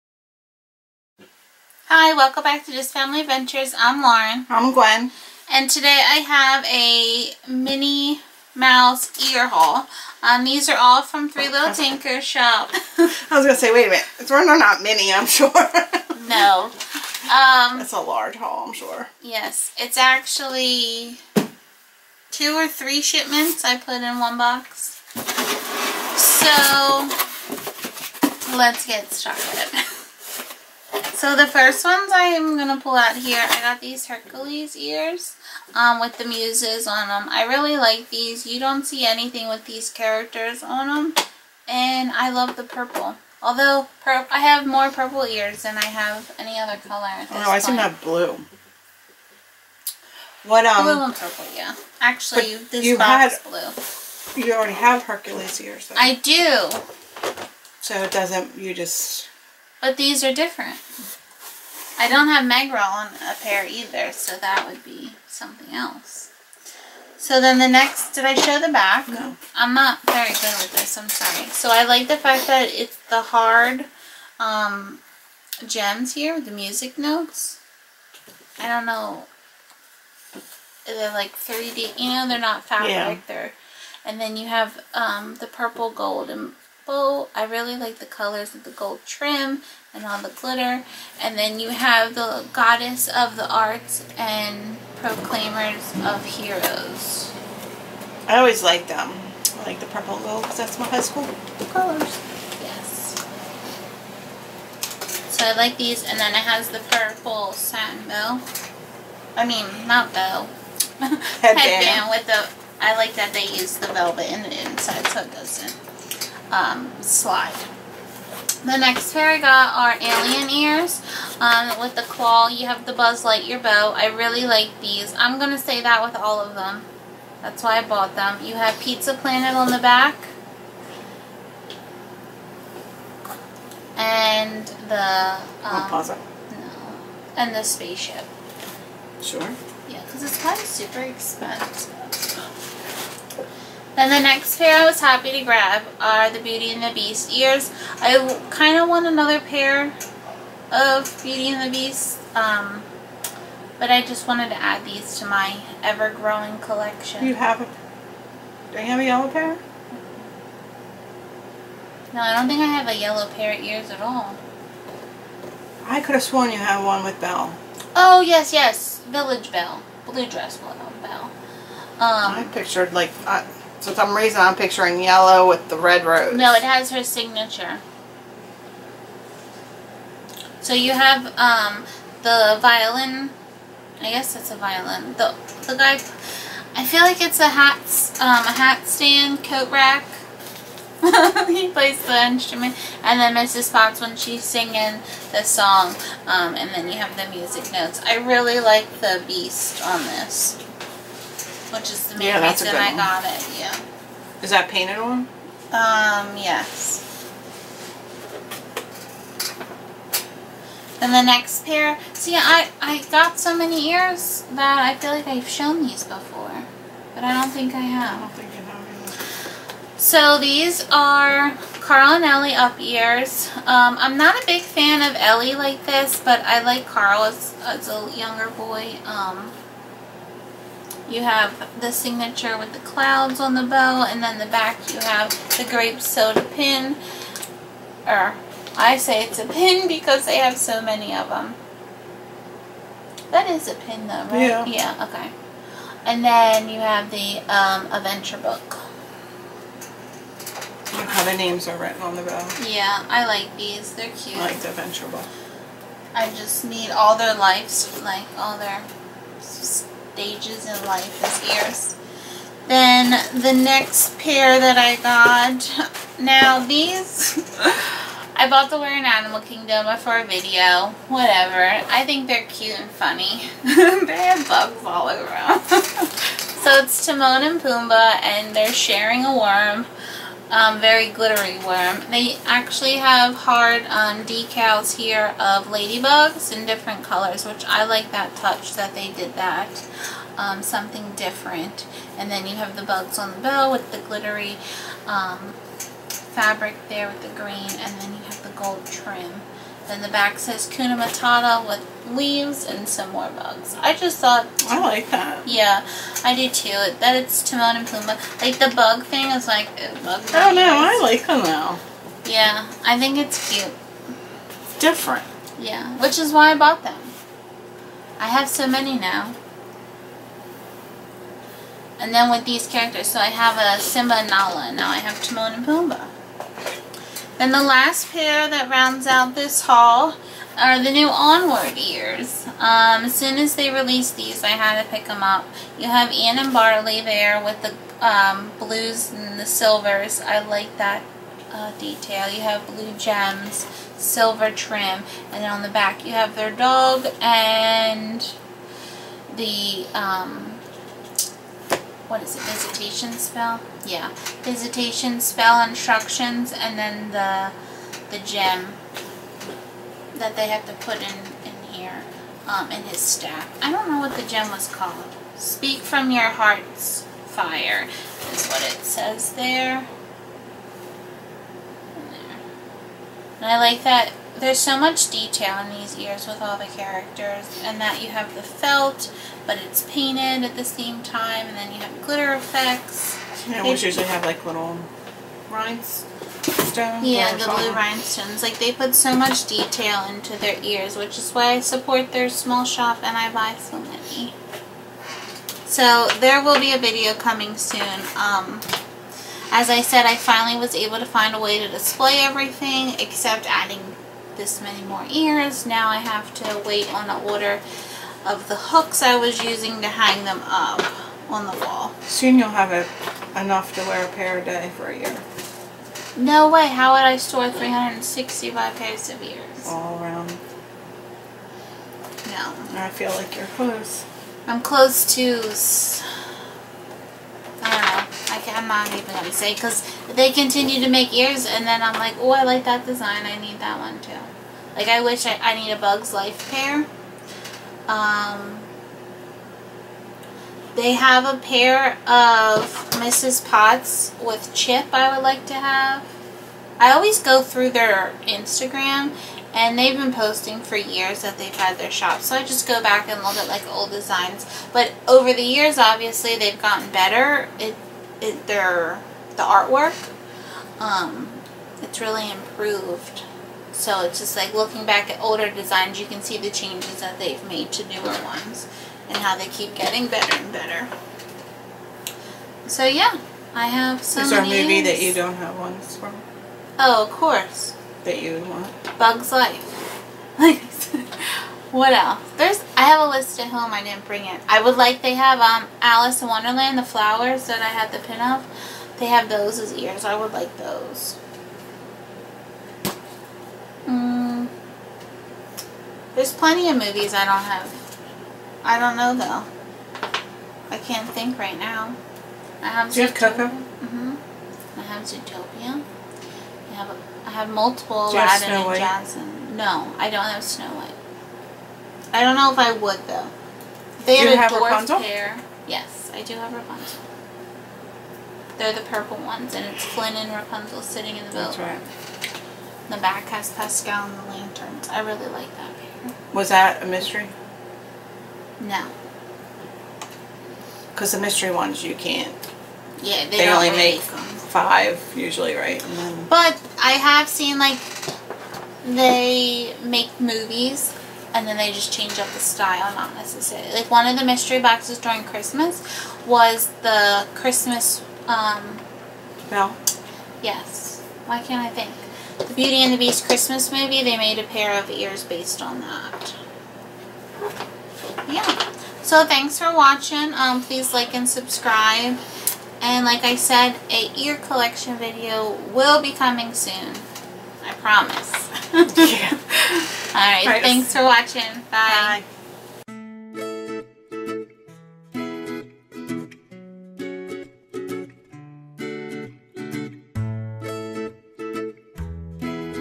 Minnie Mouse Ears Haul || Three Little Tinkers
Lauren really really loves these ears from Three Little Tinkers!!!